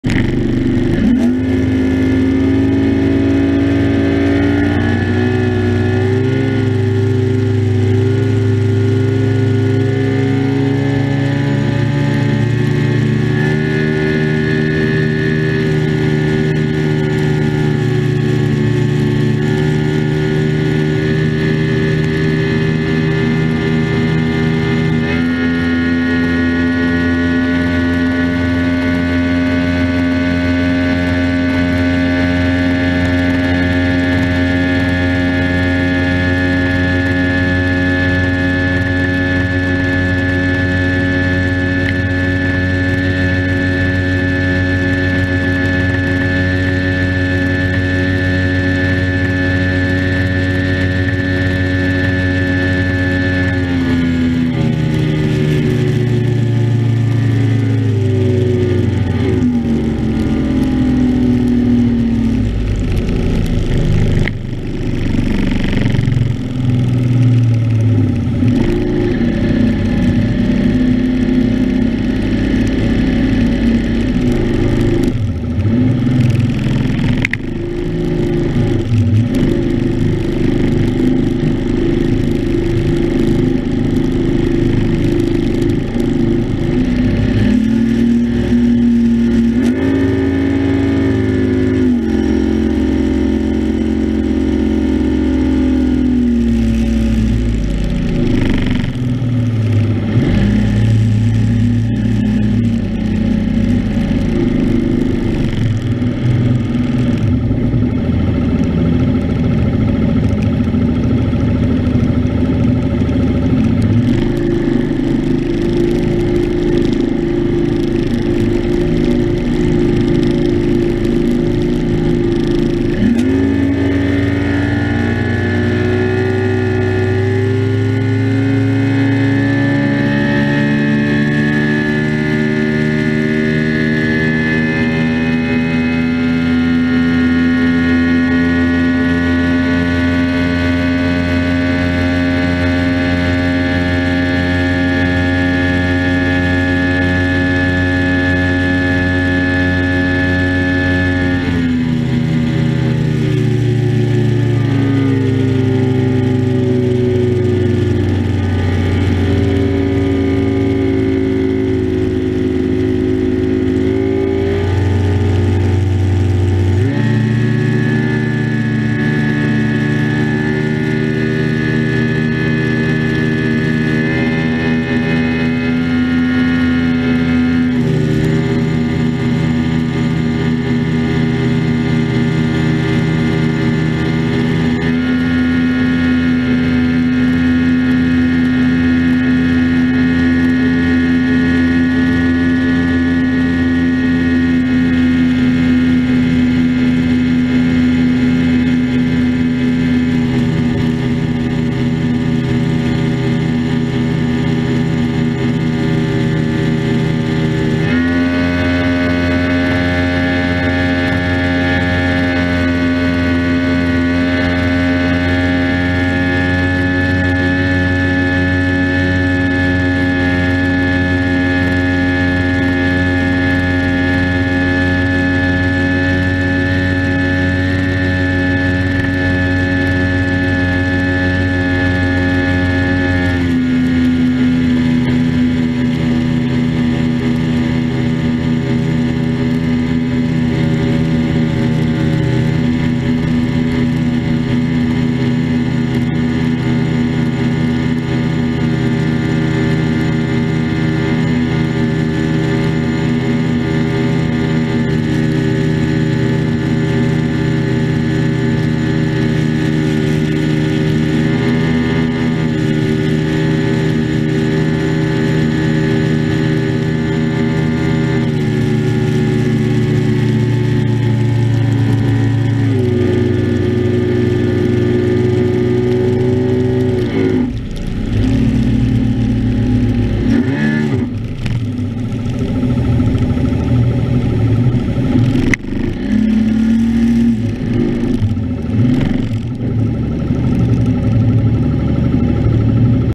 me and josh nitro z7
running a nitro z7 powered by mercury 150 on Toledo bend reservoir.